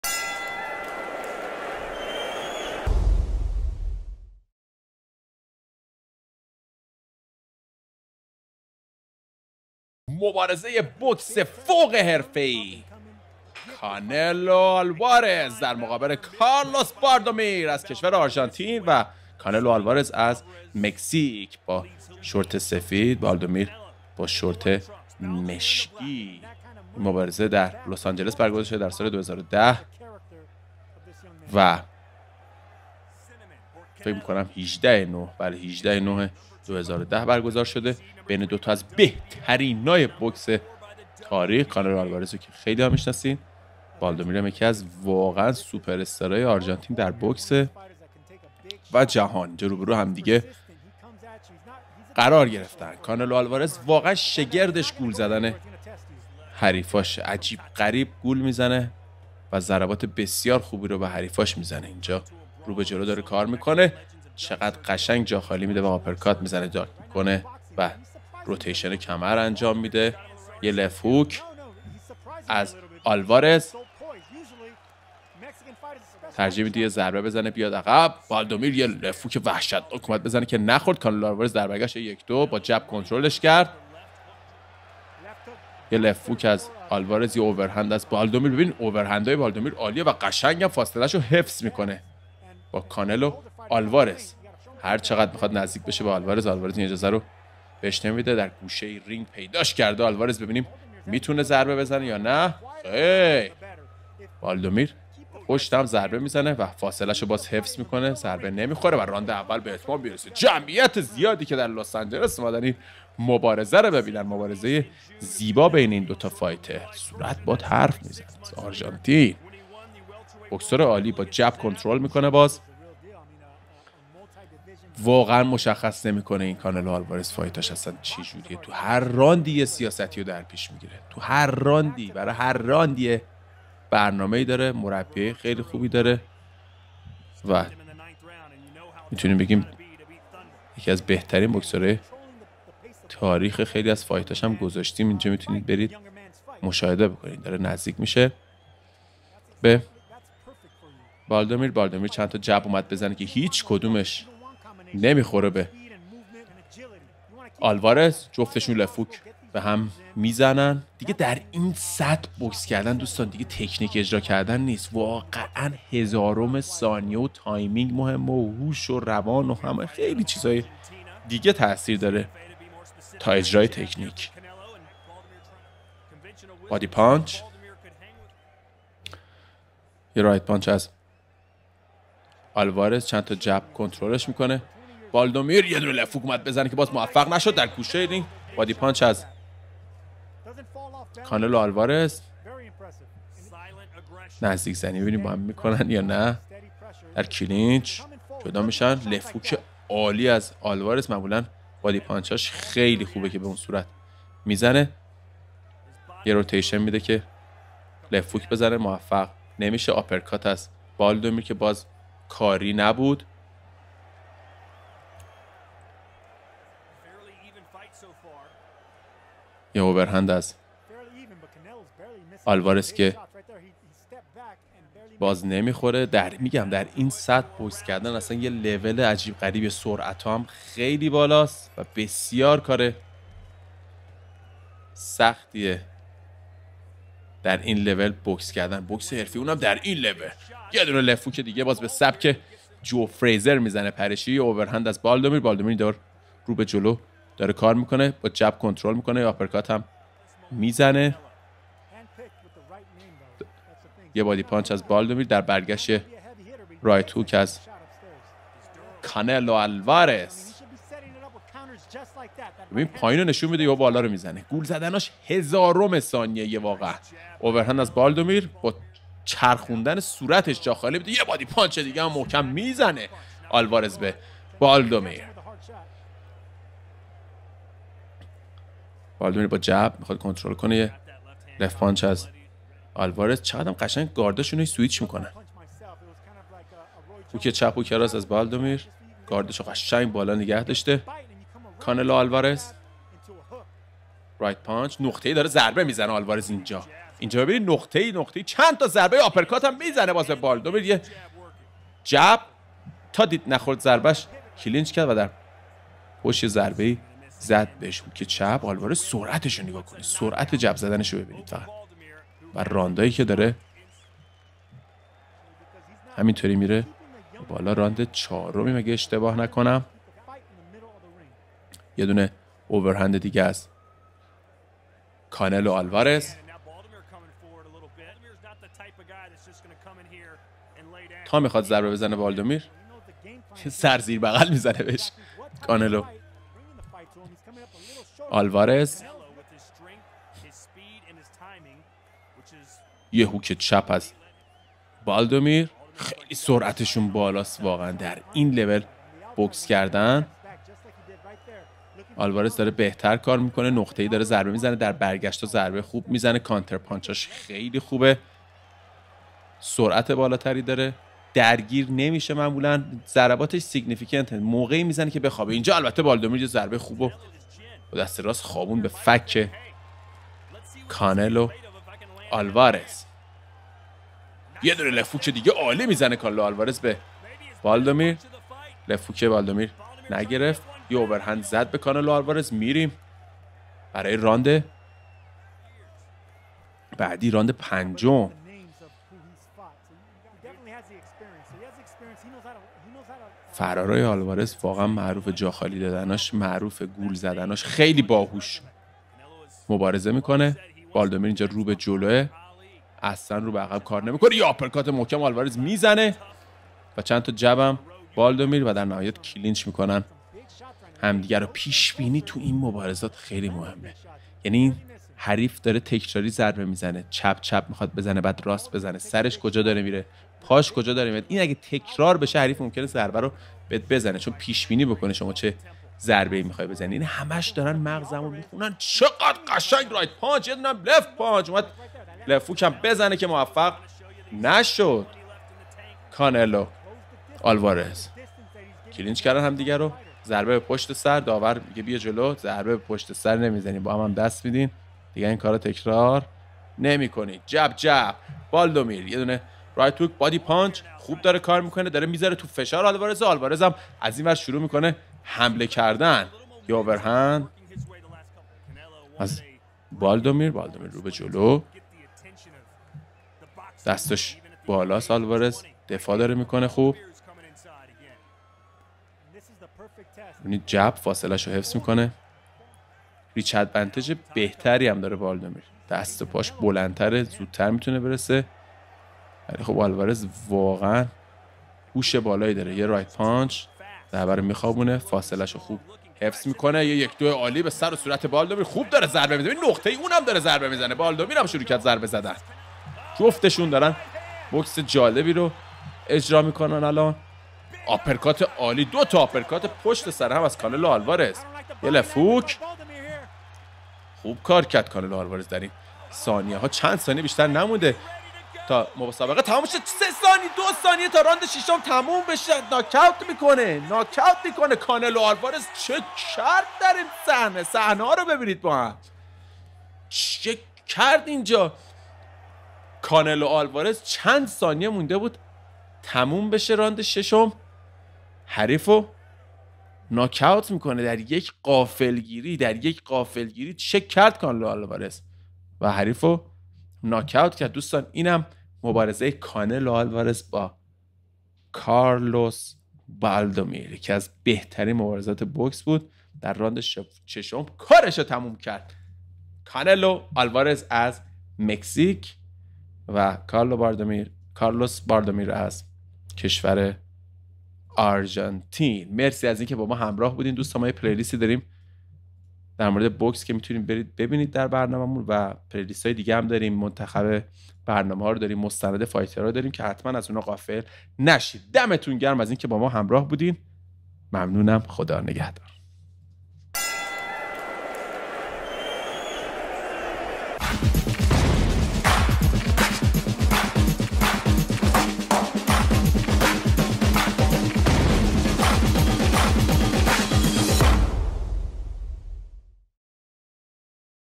بوکس حرفه ای کانلو آلوارز در مقابل کارلوس بالدومیر
اسپانسر برنامه:

 اینترنت پر سرعت و نامحدود زی تل

 This video has been edited, commented, analyzed by the MMA farsi team , with a video of myself following along the background video. A special thanks to the original content that this video was inspired by:
Linked: